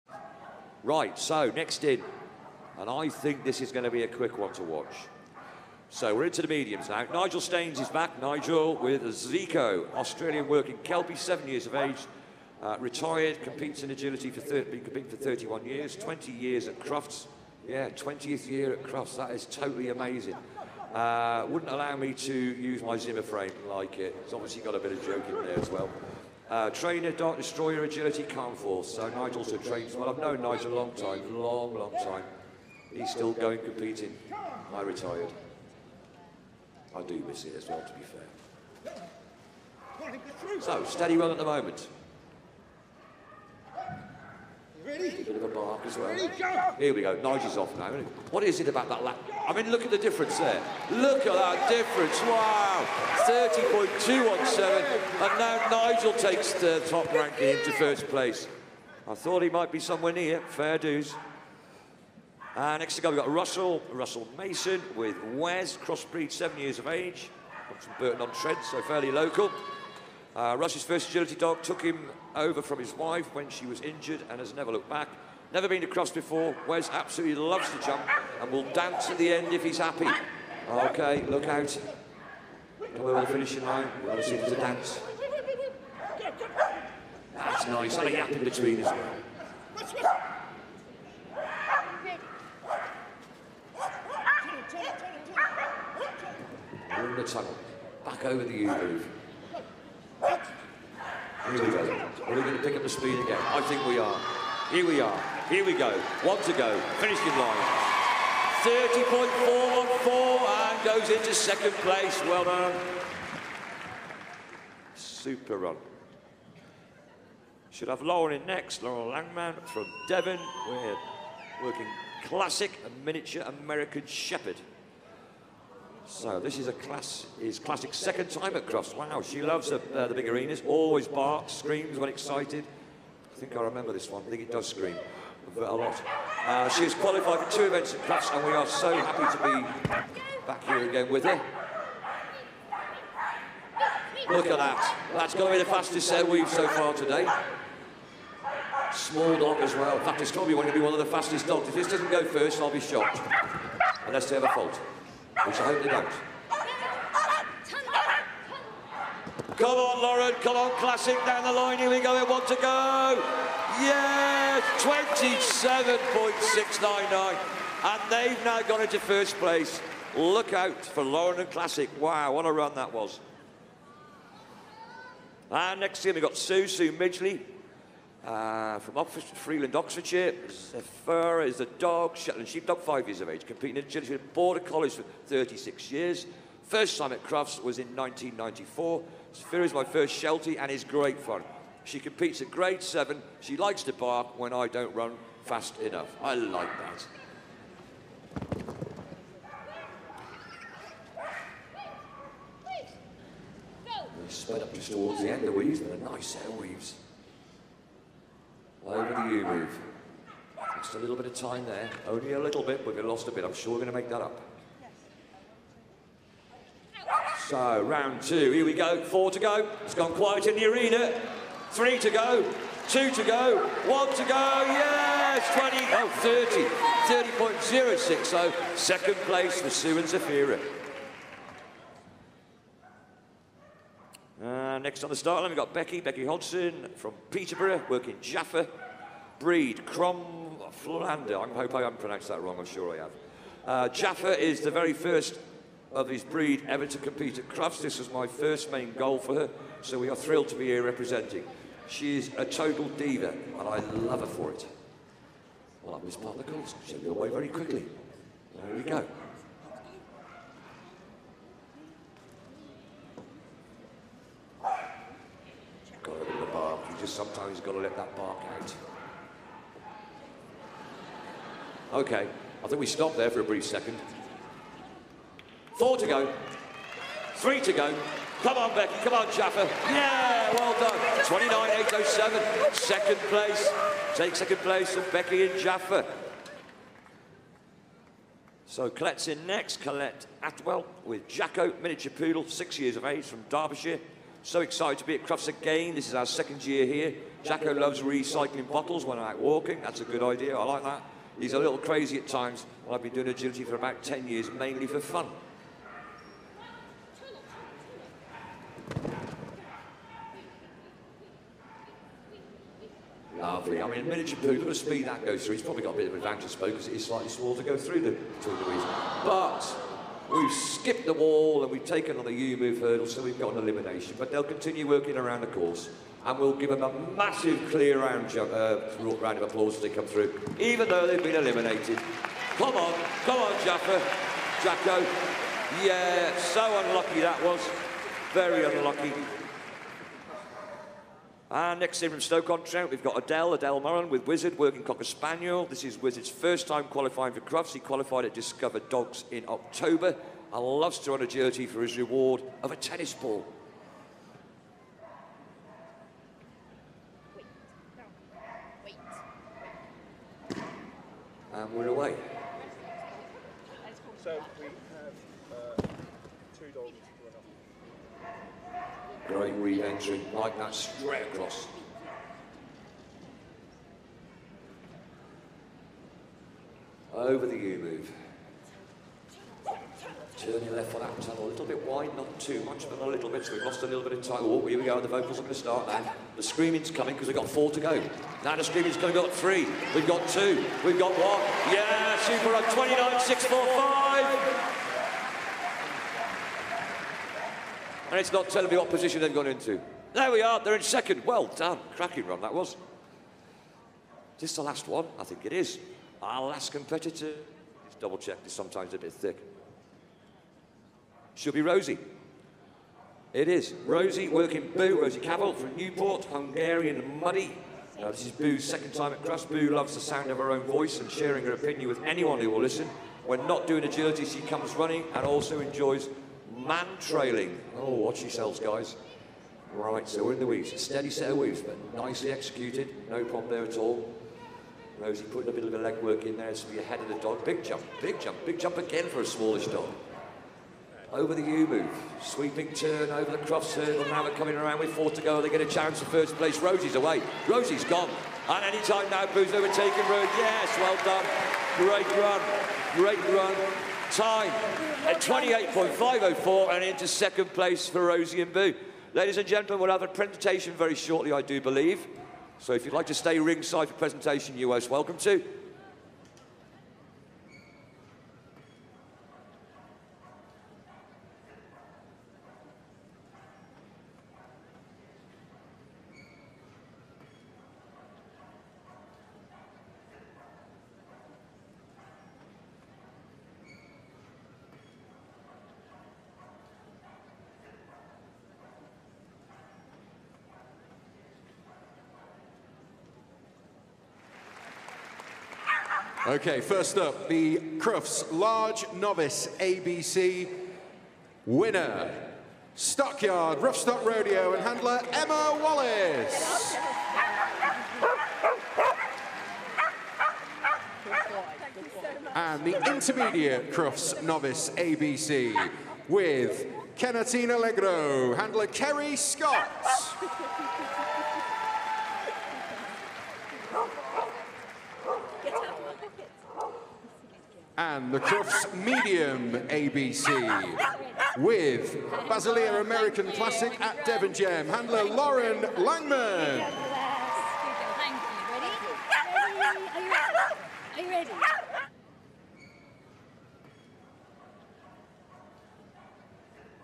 Agility - Crufts Medium ABC and Intermediate/Large Novice ABC Final (Jumping) Part 3 | ​Crufts 2023
A live replay of Crufts Medium ABC and Intermediate/Large Novice ABC Final (Jumping) from Day 2 of Crufts 2023.